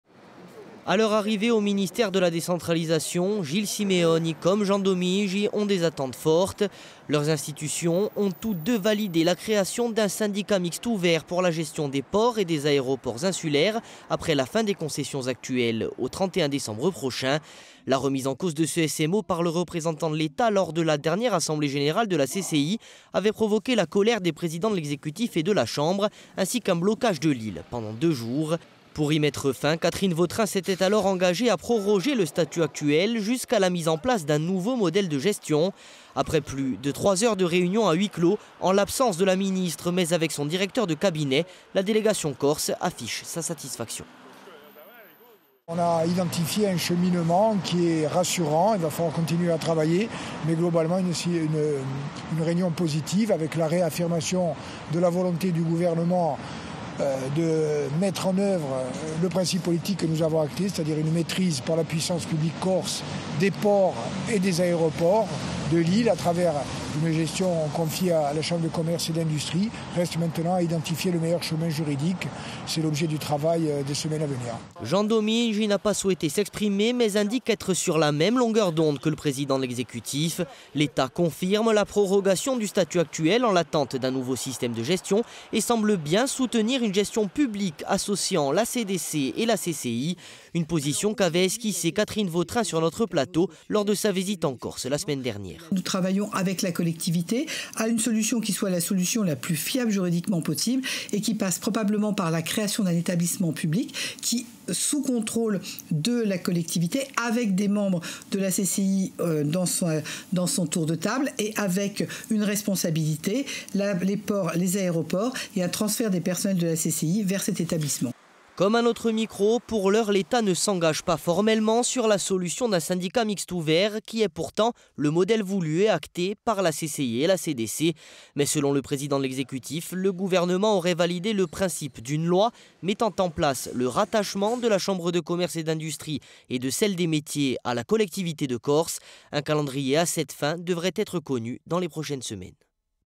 Le point sur le dossier de la gestion des ports et aéroports en Corse
Intervenants -
Gilles Simeoni et Catherine Vautrin

Equipe -
Paul Salort